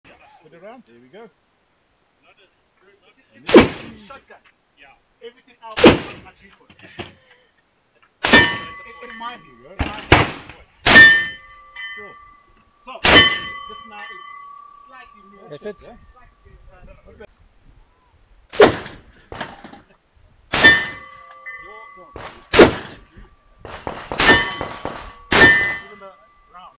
Dragunov SVD sniper rifle 7.62×54mmR At GOSA Festival
Both were hopping to get some lunch out of this
ion the end No one got lunch